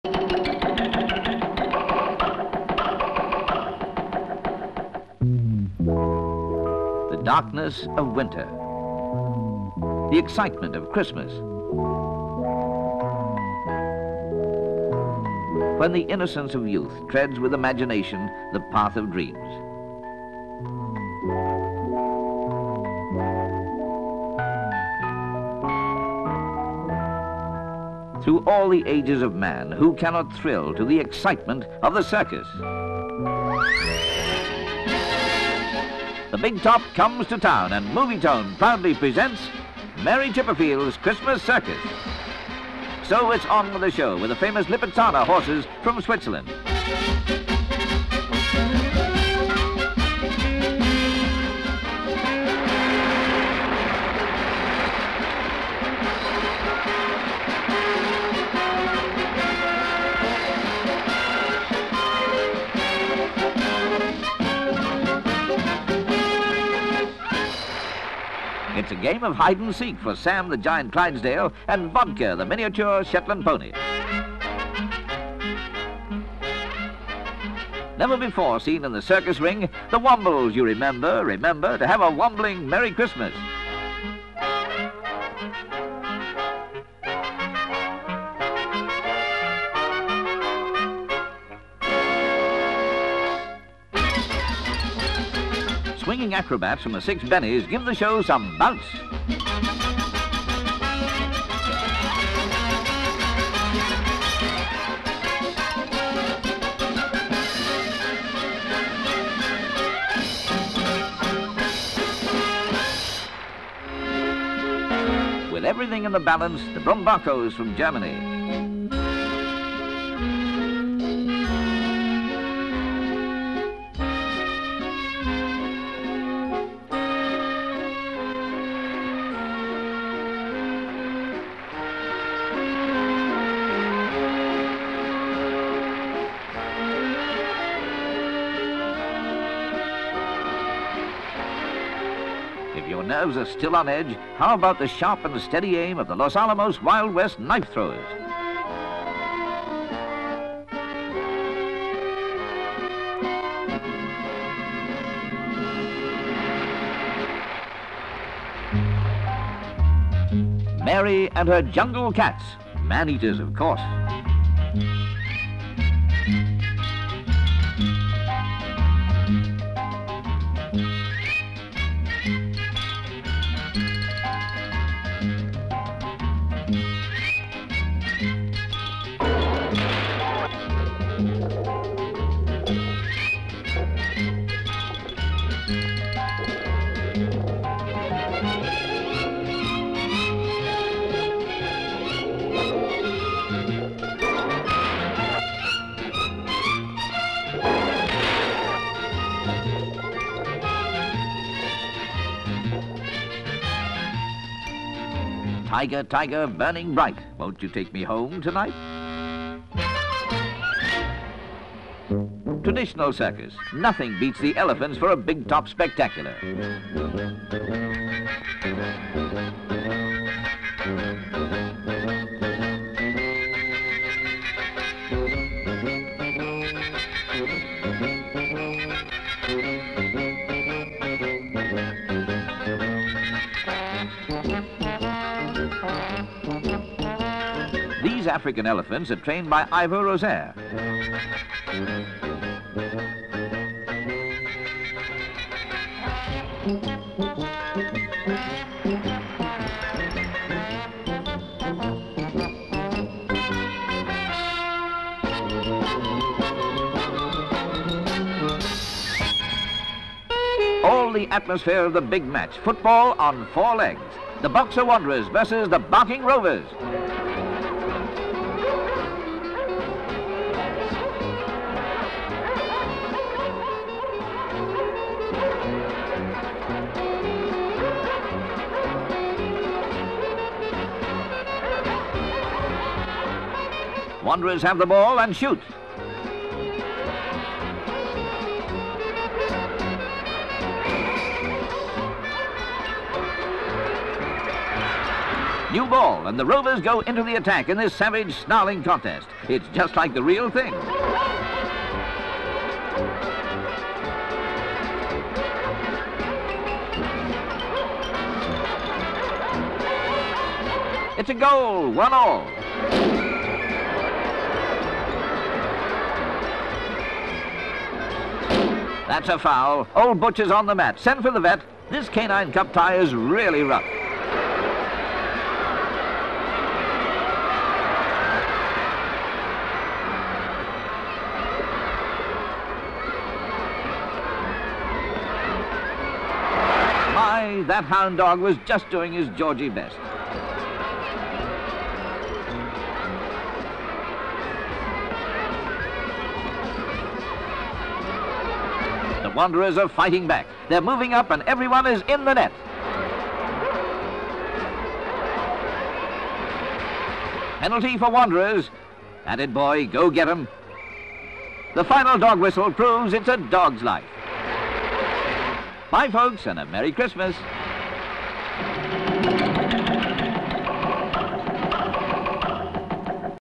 THE CIRCUS - COLOUR
(19 Dec 1974) Its the smell of grease paint and the roar of the crowd for Mary Chipperfield's Christmas Circus. All the favourites of the sawdust ring are there, the animals and acrobats, and those furry inhabitants of Wimbledon Common, The Wombles, make their circus debut. ® All the atmosphere of the big match, football on four legs, as the Boxer Wanderers take on the Barking Rovers in a Canine Cup Tie.